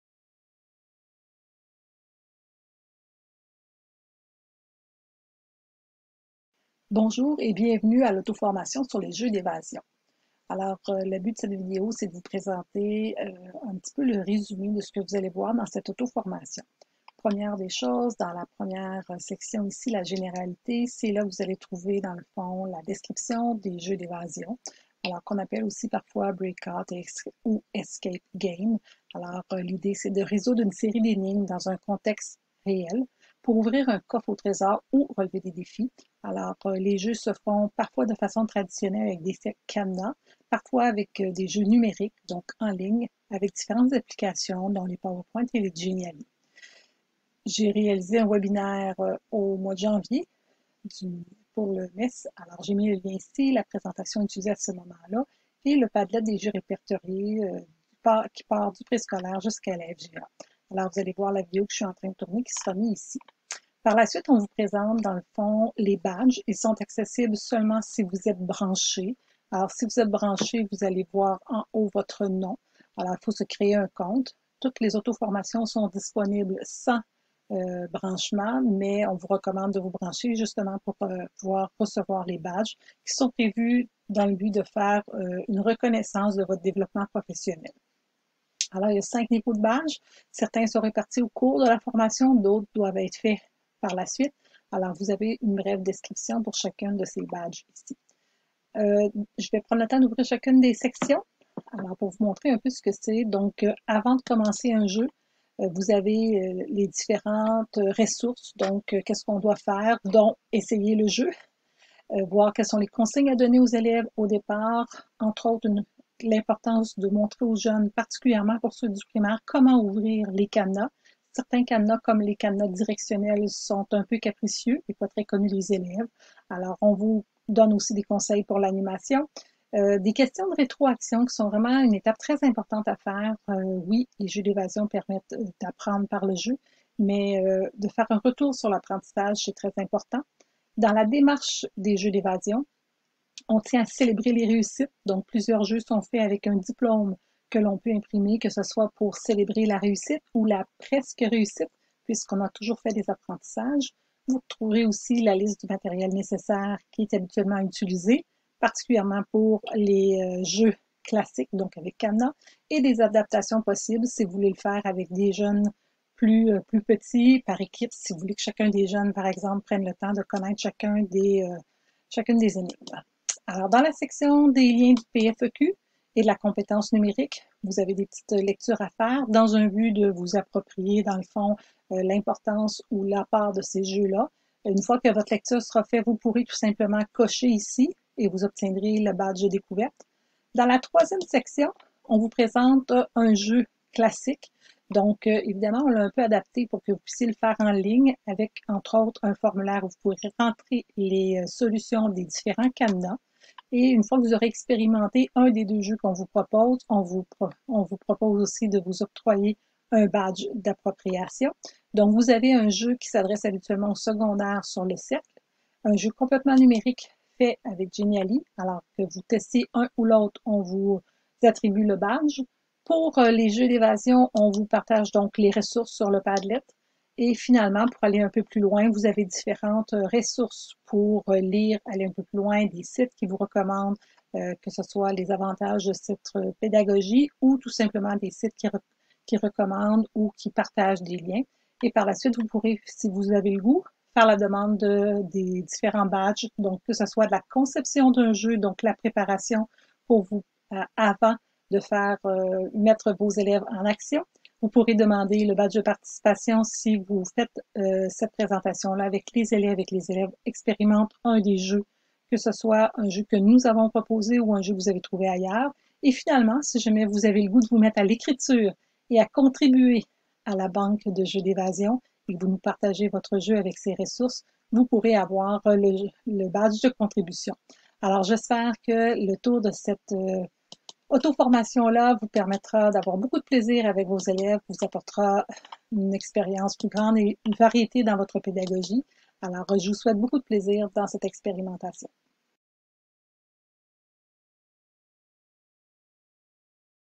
Autoformation Jeux d'évasion
Présentation de l'autoformation les Jeux d'évasion du Campus RÉCIT recitmst. Ressources qui présentent des jeux classiques avec des cadenas, numériques en ligne et d'autres qui utilisent du matériel varié.